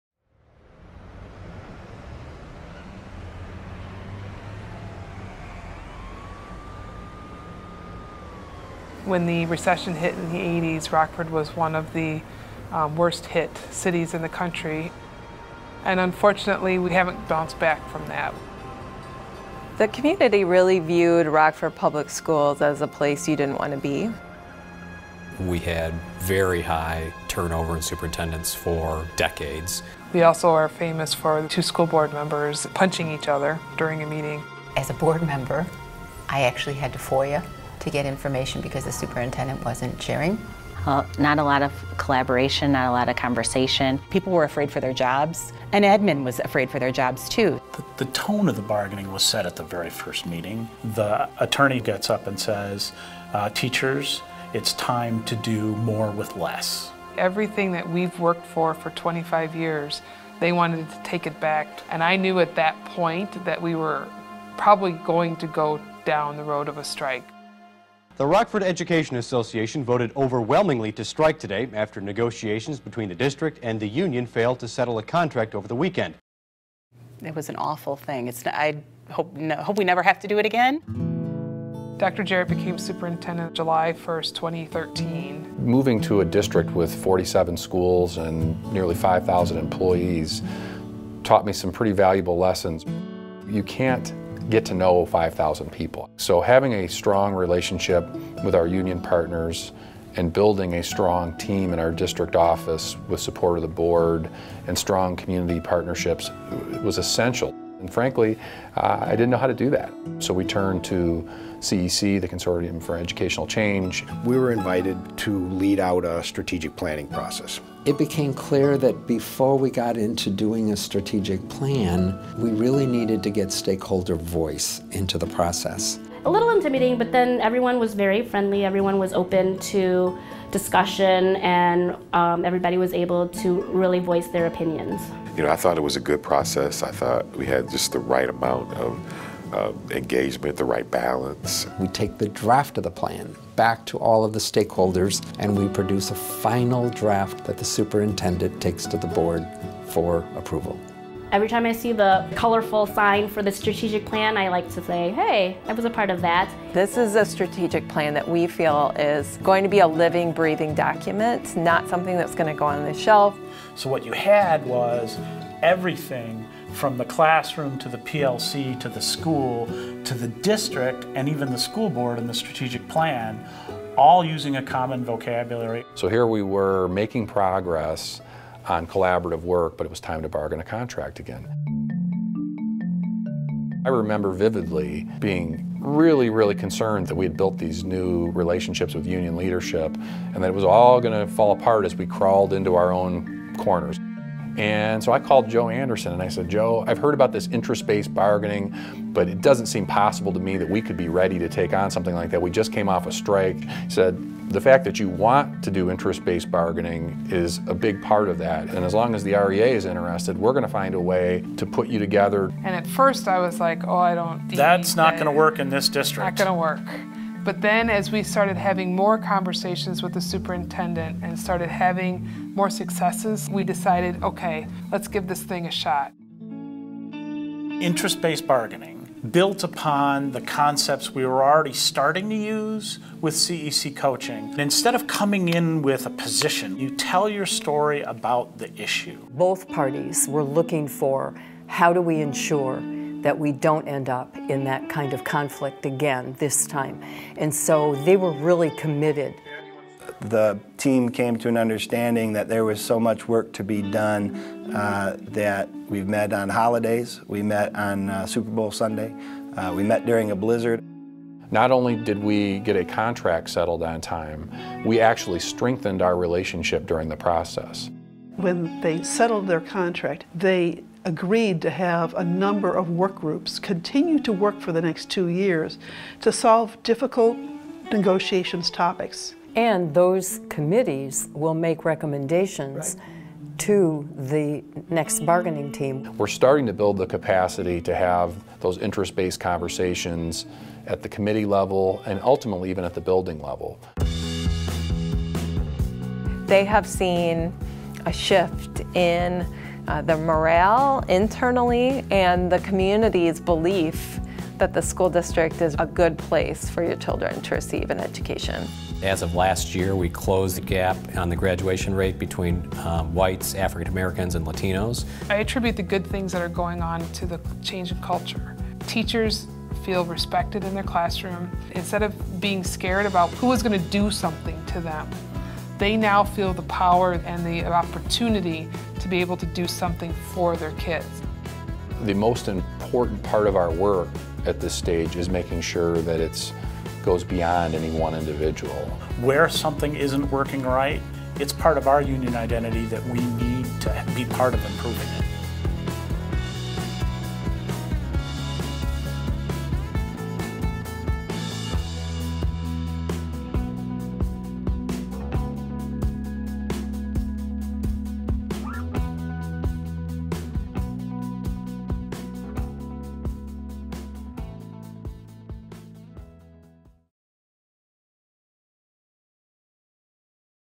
Labor Management Collaboration: Rockford's Journey
Learn how Rockford Public Schools had positive impacts on the district’s culture and student outcomes through a comprehensive partnership approach with the Consortium for Educational Change (CEC). Rockford Public Schools and CEC partnered on new approaches to collaboration and collective bargaining, stakeholder engagement in major initiatives, and relationship building among the teacher union, administration and the Board of Education.